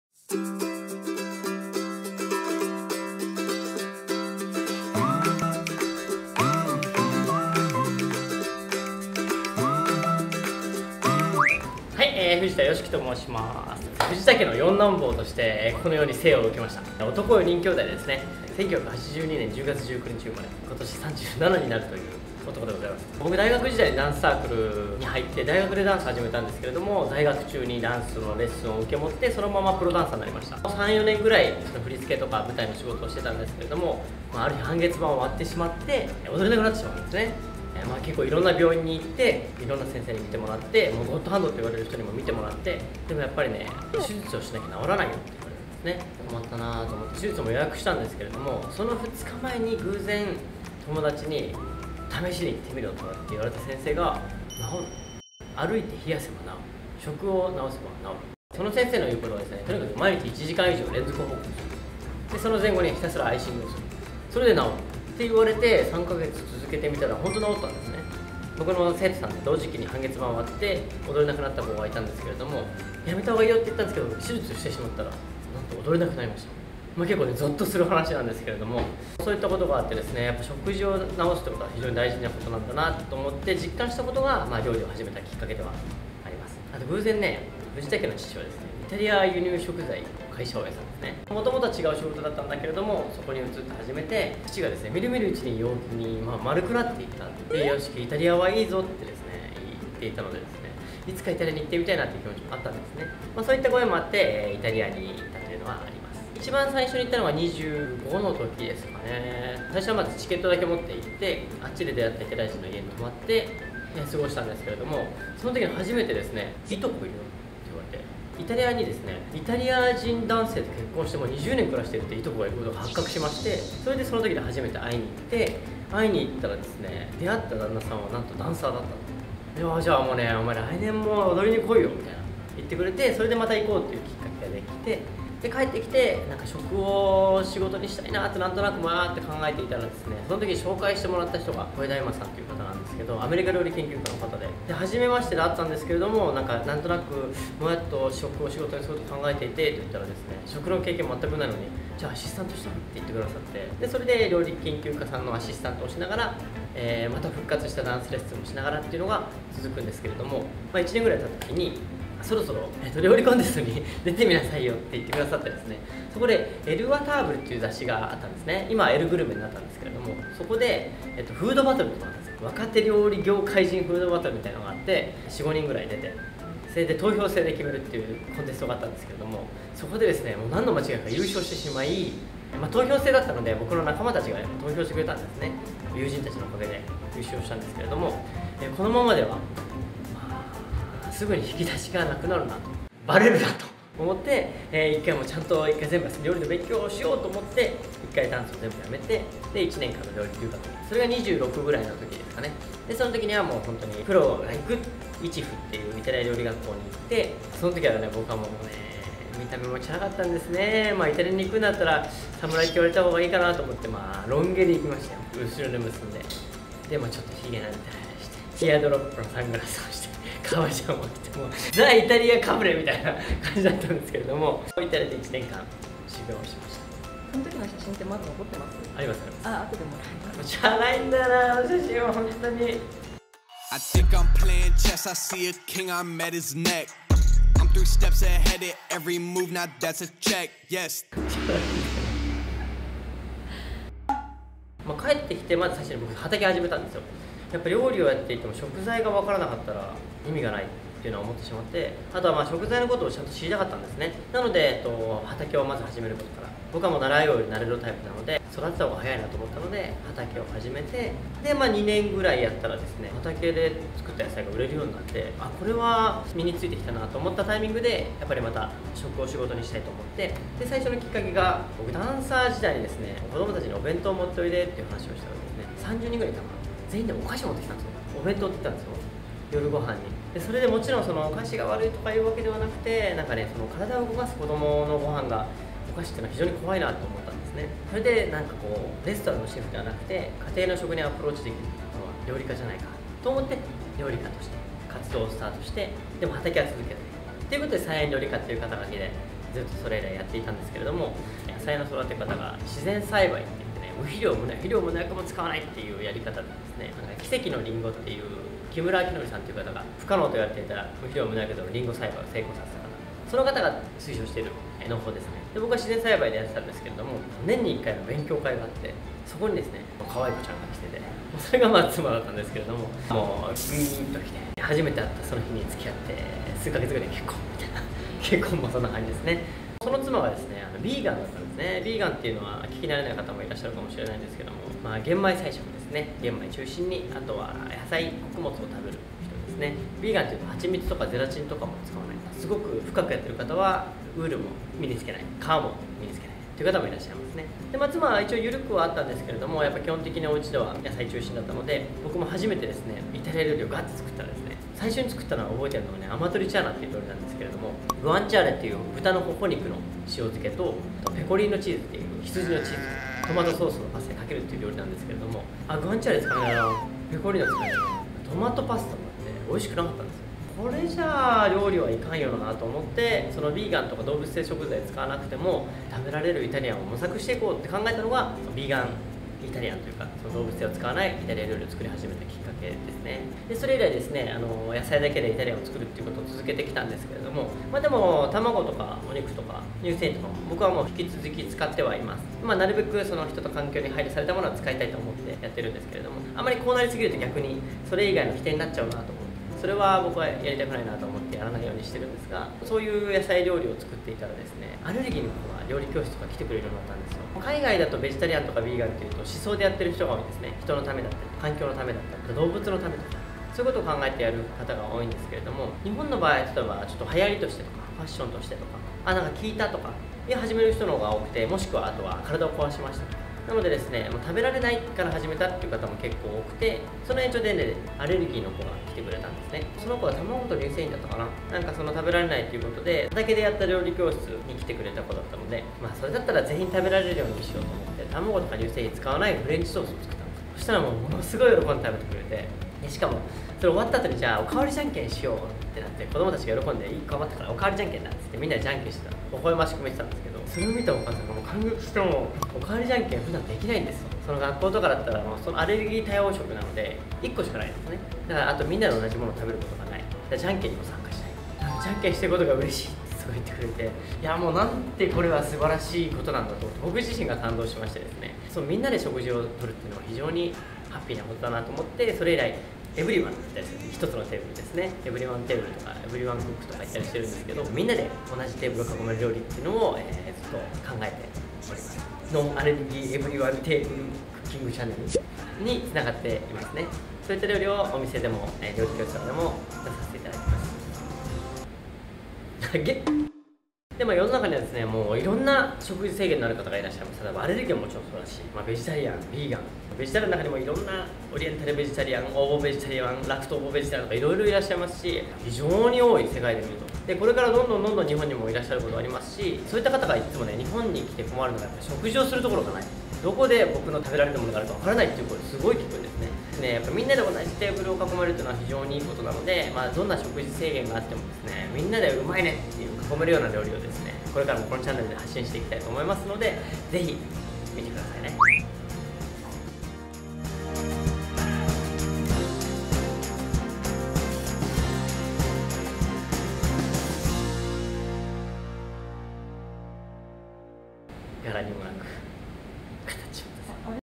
【実話】プロダンサー ▶︎ ｲﾀﾘｱ留学 ▶︎ 料理人 ▶︎ 菜園料理家 ▶︎ 新人YouTuber ← new!!【最後は真面目な話】
ここは 転職を つかさどる
ダーマの神殿。
どの 職業に なりたいのかね？

｜　 プロダンサー　　　　　　　　　  ｜
｜▶︎ ザ・イタリアかぶれ　　　　　　  ｜
｜　 菜園料理家           　　　　　　　   ｜
｜　 YouTuber　　　　　　　　　　　 ｜

YOSHIKI's NO アレルギー Everyone Table Cooking Channelの
よしきです！！！

今回は！長らくの謎！よしきって何者？にお答えします！
ユーチューバーになって何がしたいのか？から
奥さんとの馴れ初めまで！
まずは半月板を割るところから！（？）

今回の動画の一番の謎はよしきが半袖を着ているところですね！チャラ!!

【よしきがシェフをしているらんどね空と海のHP】